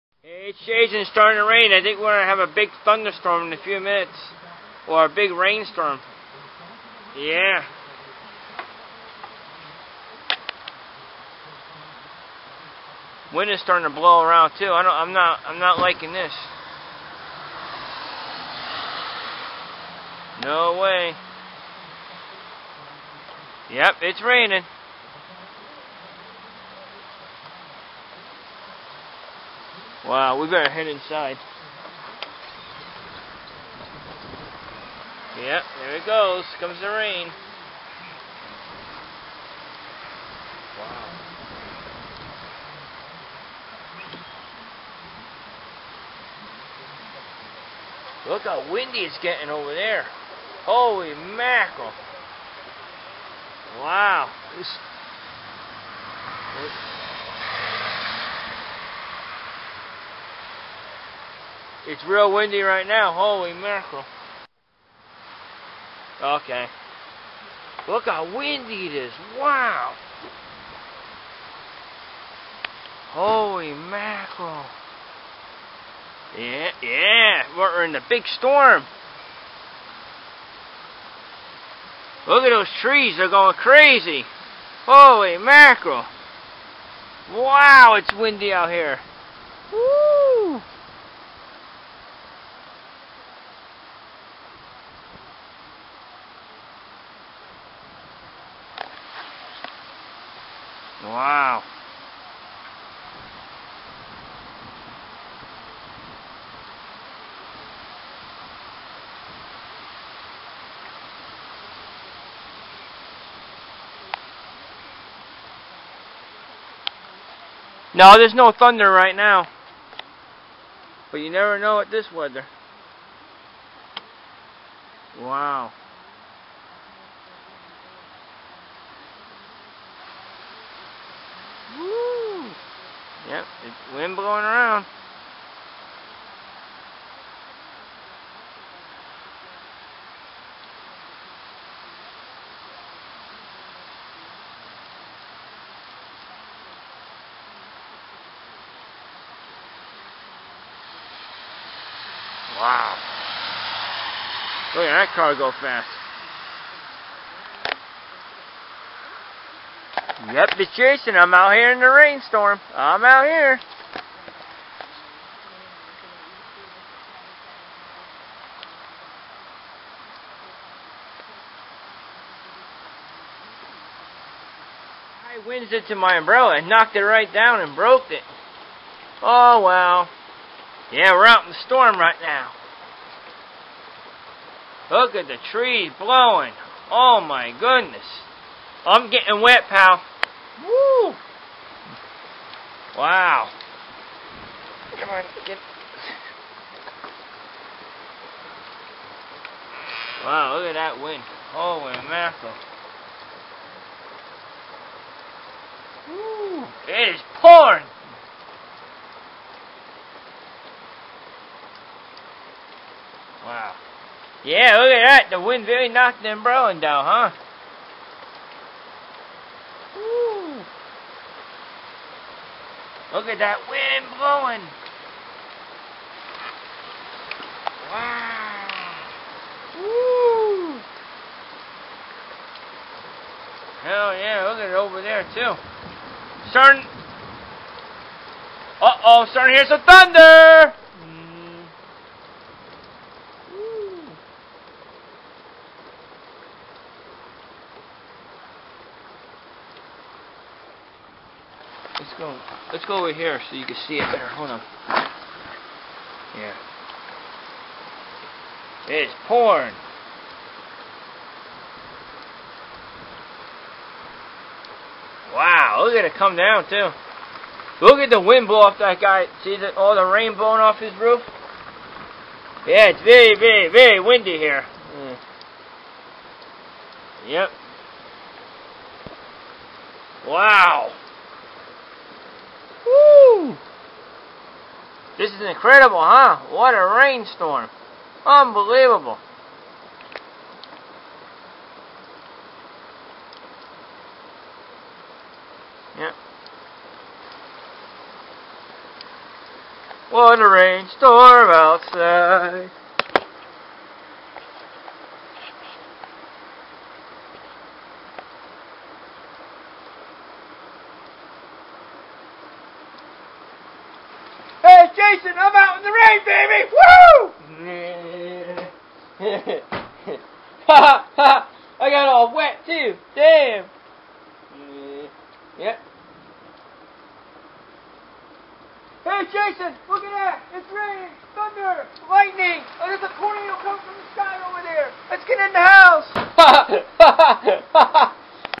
big rain storm on june 28 2010
its raining outside by jason.